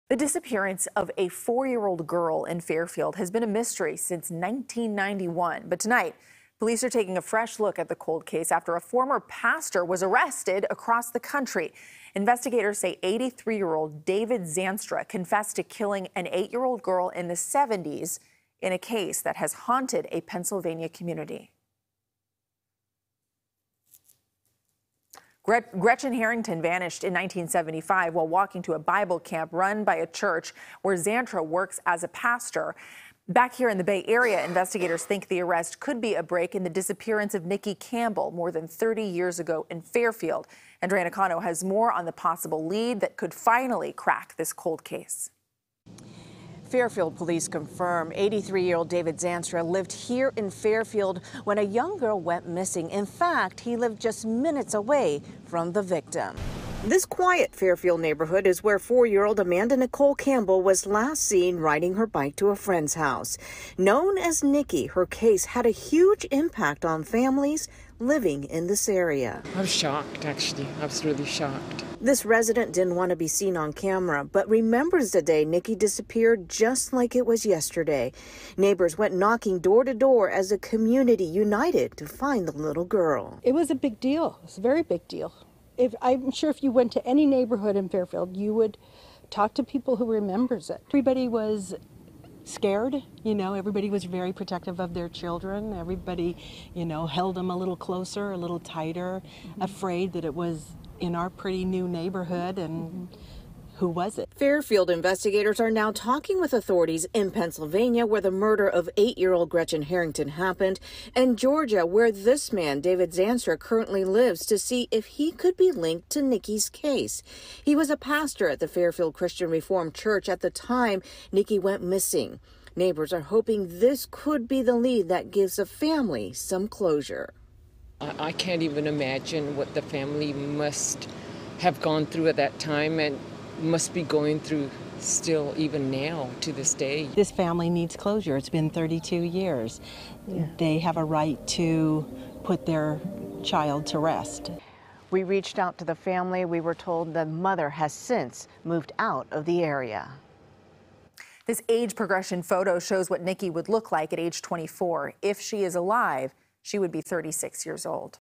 Pennsylvania pastor charged in 1975 murder could be linked to missing Fairfield girl's cold case
Andrea Nakano reports on Fairfield police reopening a 1991 cold case of a missing girl that could be linked to